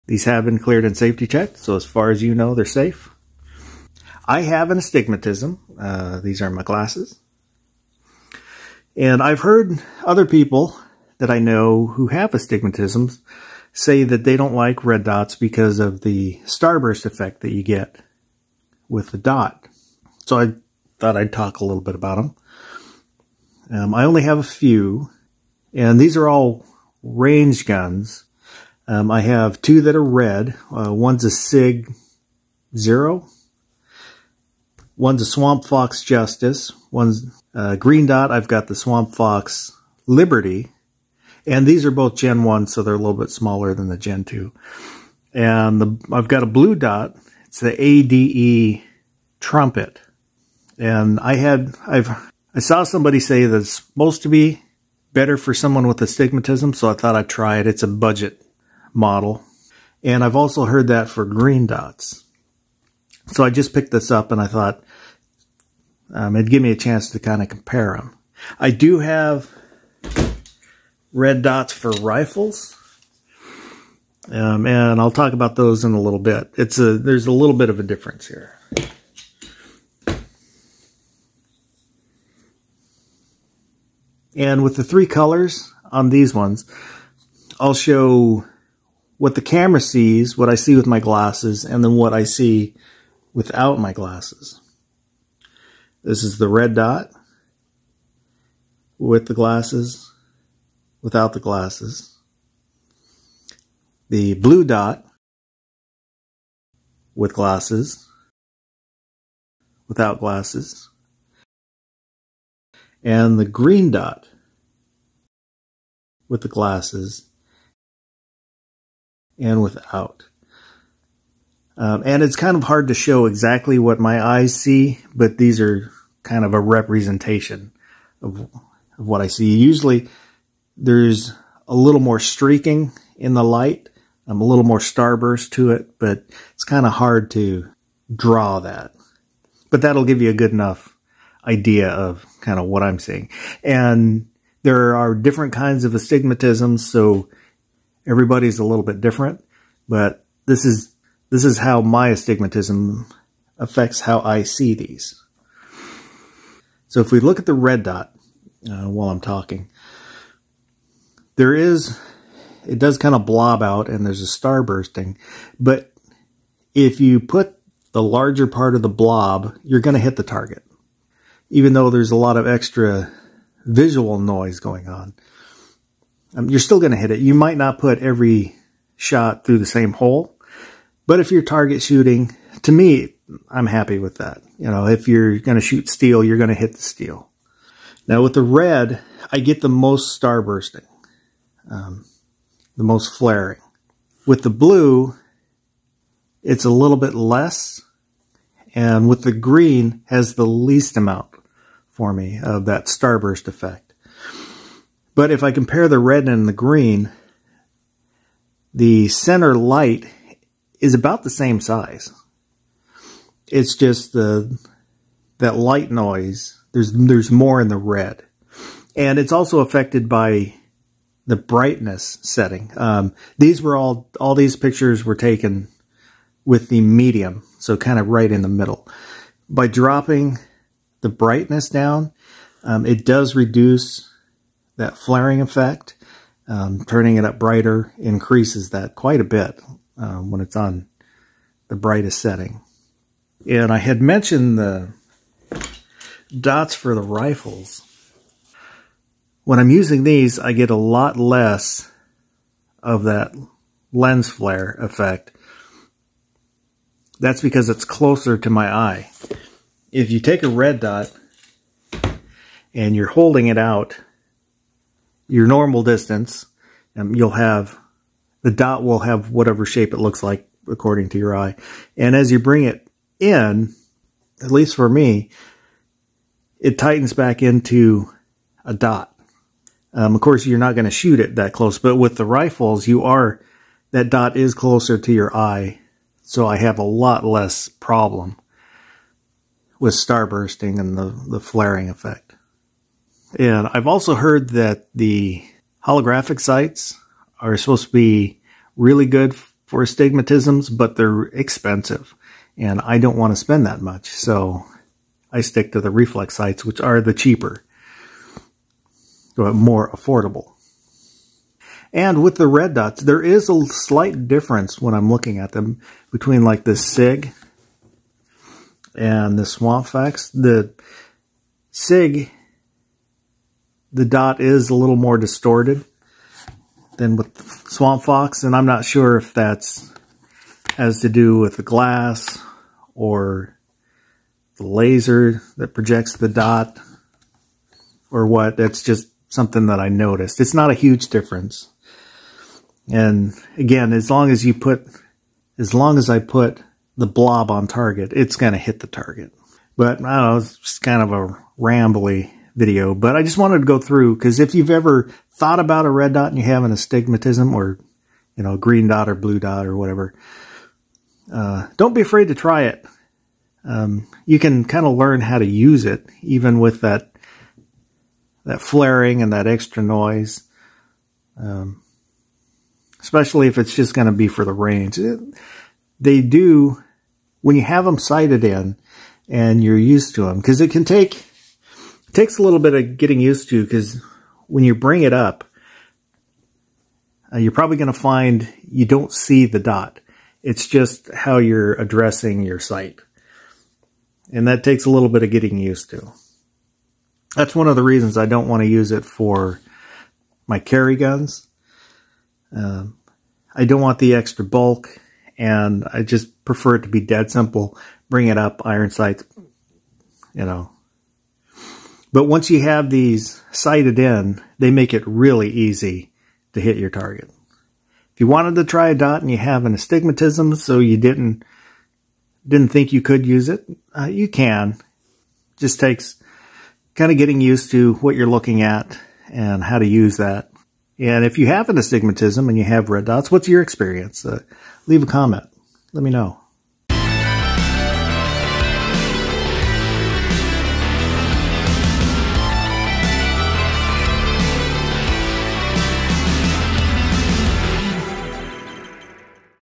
Reflex Sight Colors And Astigmatism
Looking at how my astigmatism affects how I see red, blue and green color reflex sight dots.